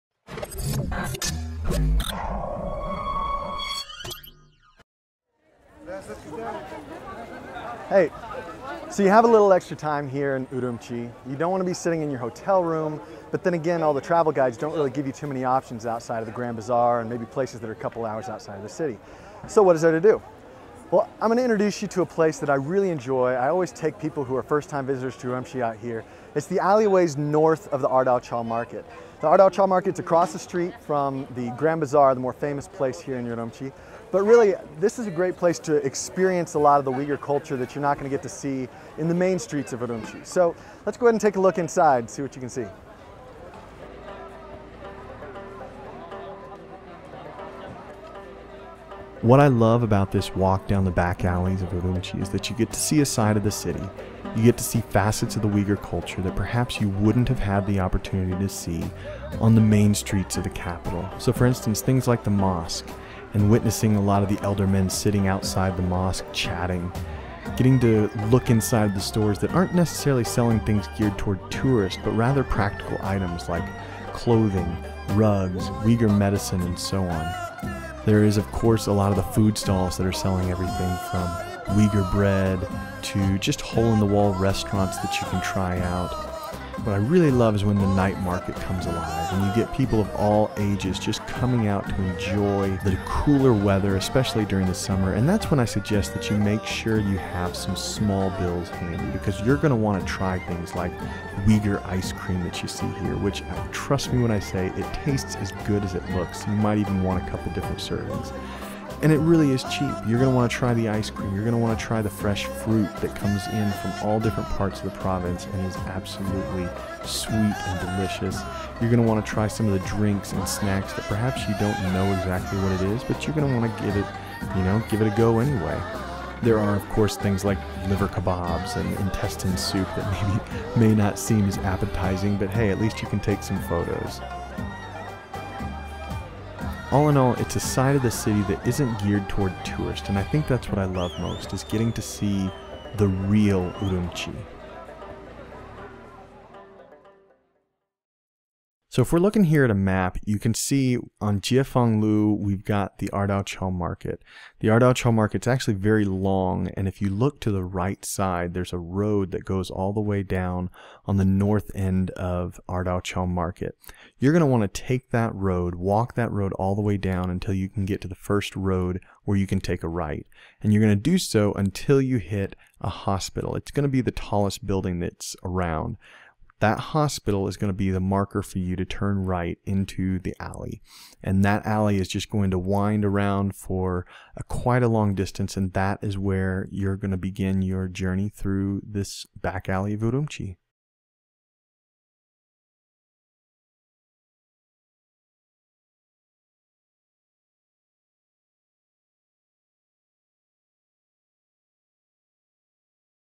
Urumqi Walking Tour | Uyghur Culture in Xinjiang's Capital (UPDATE: Now Destroyed)
NOTE: Unfortunately, this beautiful Uyghur alley in Urumqi has now been torn down, so this video stands as a record of what used to be. I hope you'll still enjoy.

Welcome to the first walking tour of Urumqi (乌鲁木齐), something that anybody who is traveling to Xinjiang will want to do. It starts at the ErDaoQiao market (二道桥) across the street from the Urumqi International Grand Bazaar (乌鲁木齐大把杂) and ends up near the Urumqi NanMen (南门).